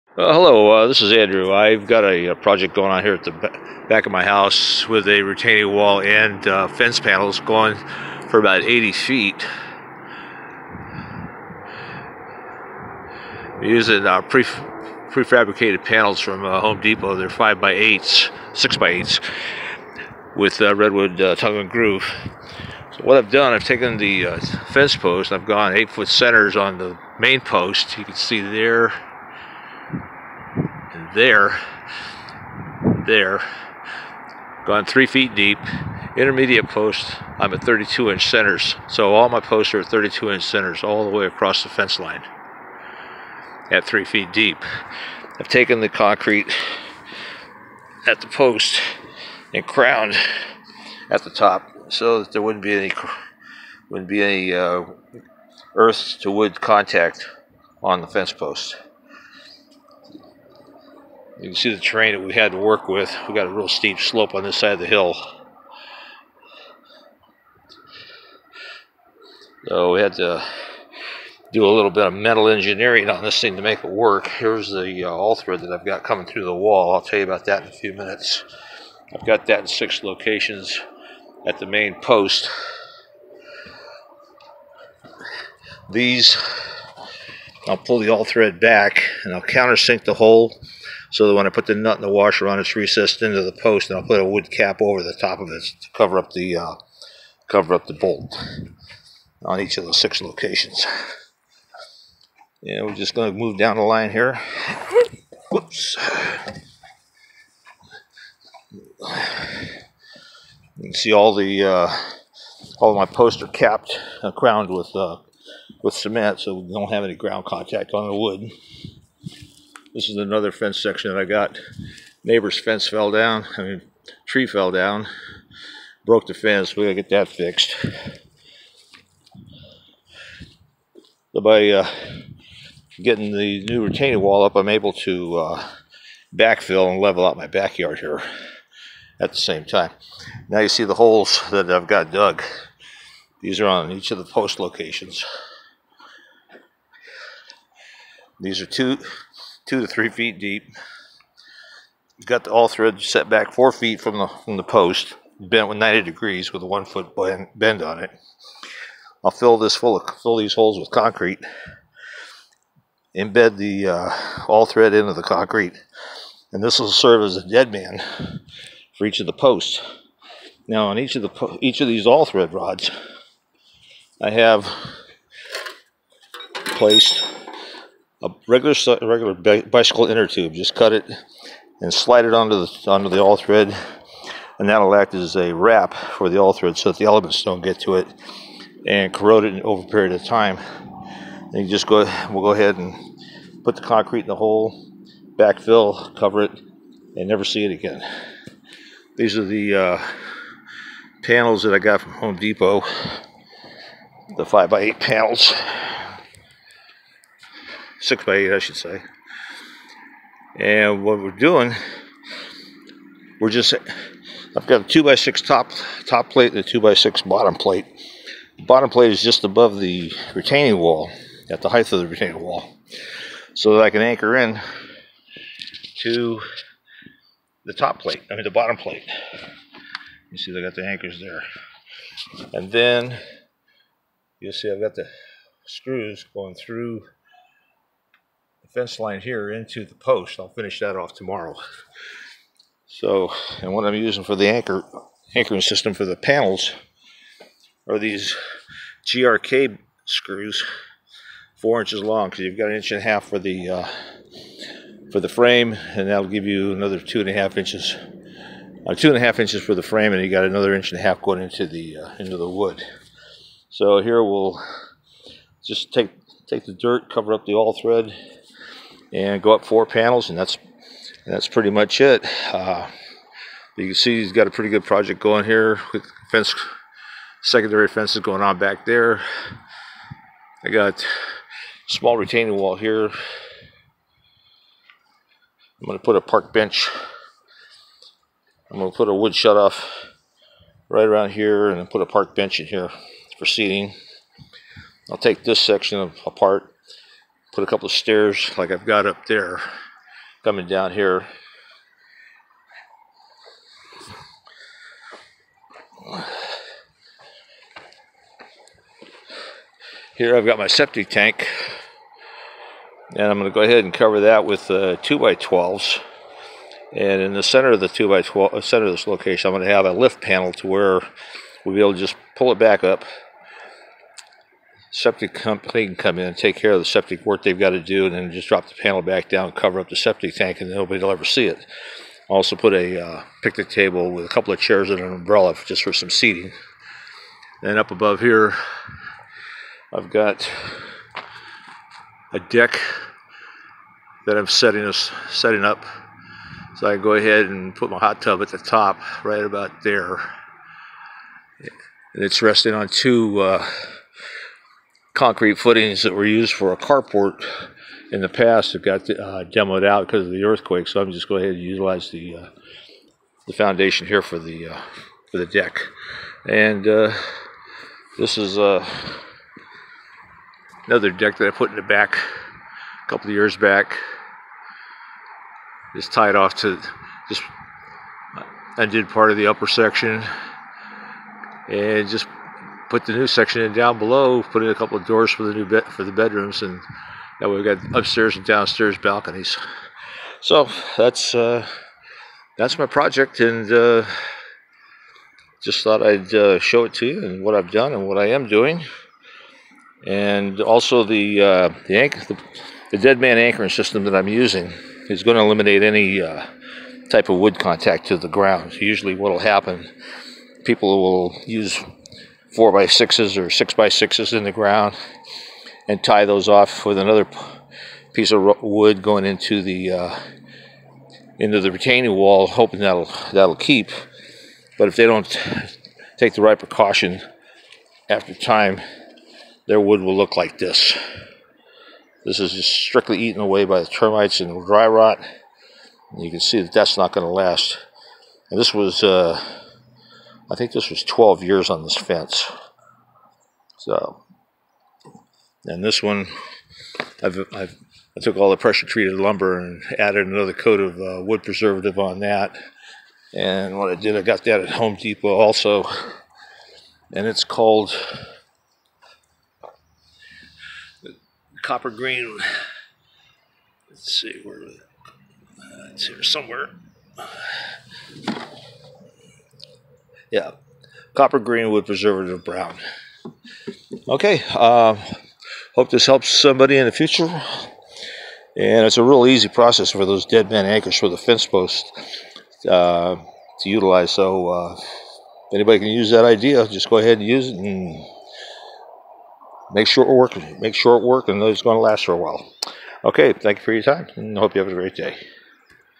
Tie-Back retaining wall system
Simple, low cost and effective Tie-Back retaining wall system using post cement, bent all-thread with re-bar rod and bicycle inner tube.